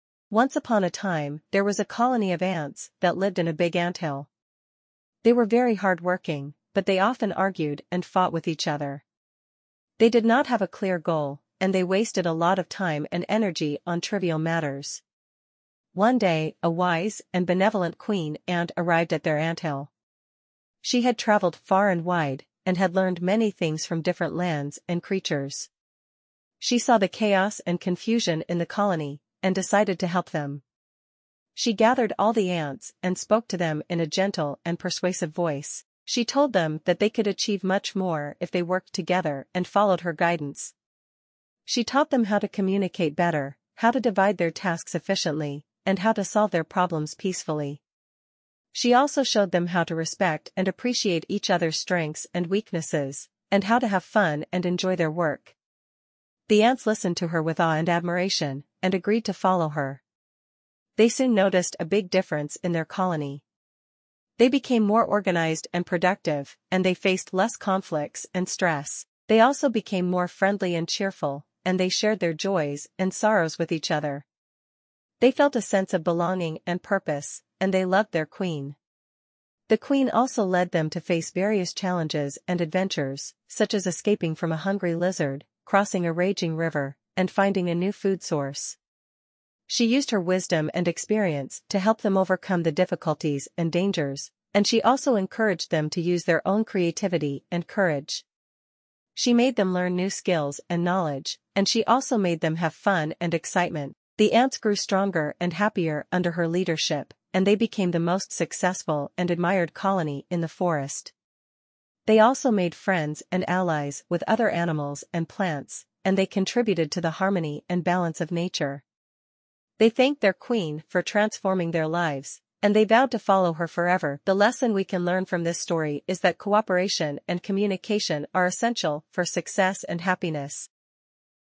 The Ants and the Queen: A Story of Cooperation and Communication
The ants improved their lives by working together and following the guidance of their wise and benevolent queen. They also learned how to respect and appreciate each other and how to have fun and enjoy their work. They became stronger, happier, and more harmonious as a colony. 🐜